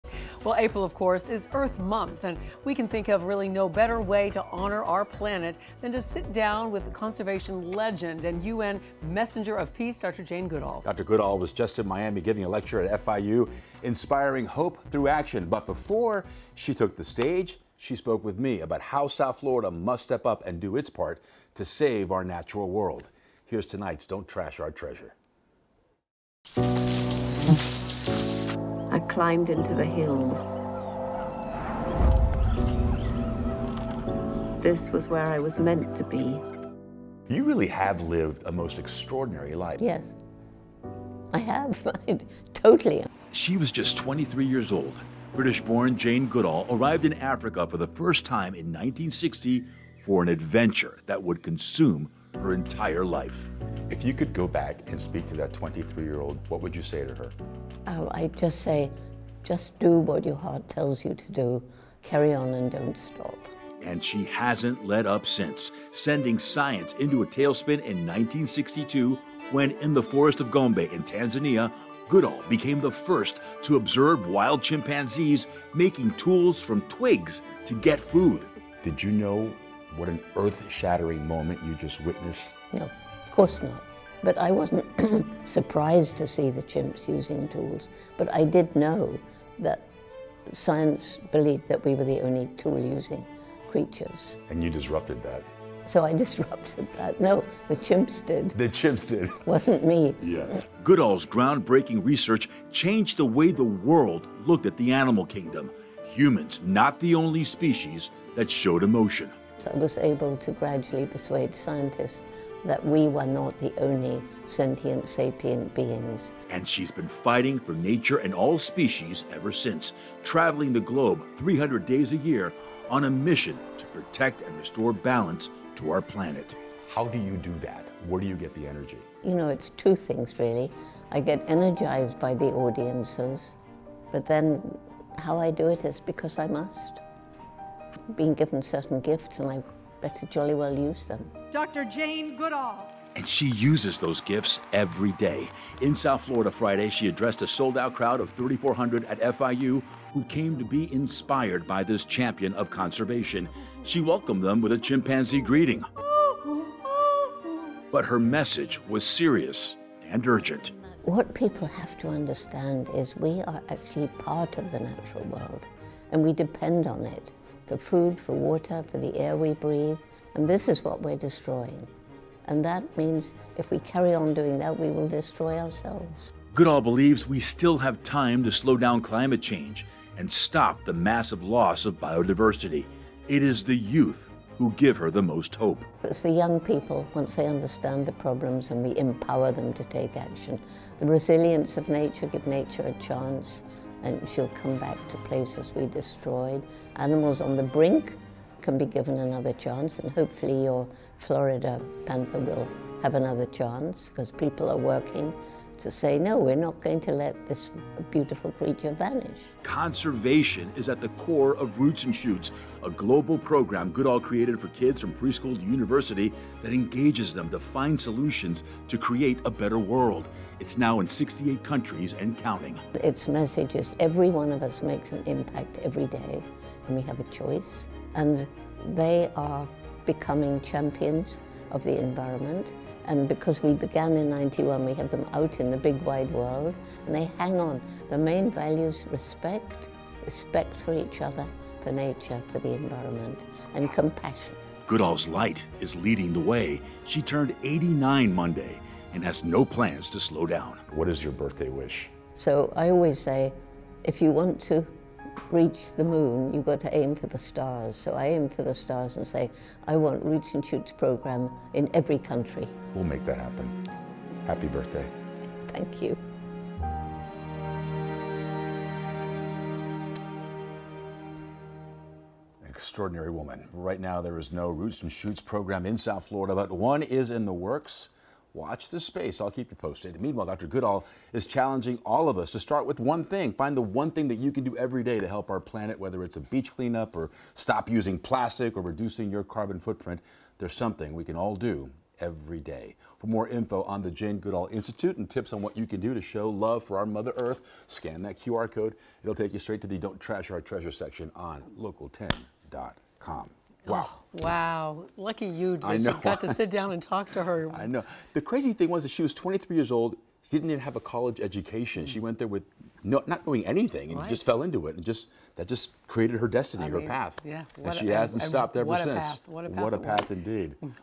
Jane Goodall sits down with Local 10 News to discuss what South Florida can do to help environment
April is Earth Month, and we can think of no better way to honor our planet than to sit down with conservation legend and United Nations Messenger of Peace, Jane Goodall.